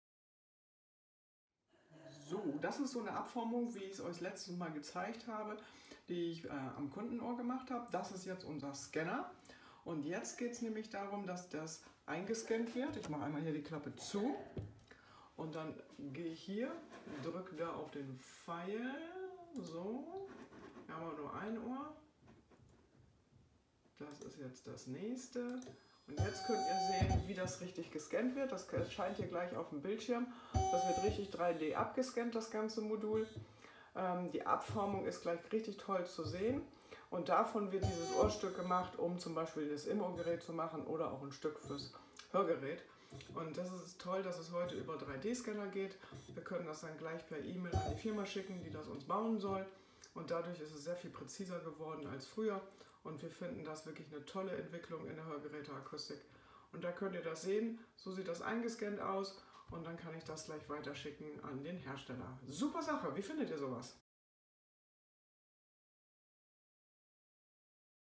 3D Scanner in der Hörgeräteakustik, neuste Technik hält Einzug, es wird immer moderner
Die Abformungen  werden heute direkt beim Hörgeräteakustiker mit Hilfe eines 3D Scanner digitalisiert und dann sofort per Mail weitergeleitet. Es ist wesentlich genauer und viel schneller, weil wir keinen Postweg mehr haben.
Neuste Technik in der Herstellung von Ohrstücken für unsere Kunden.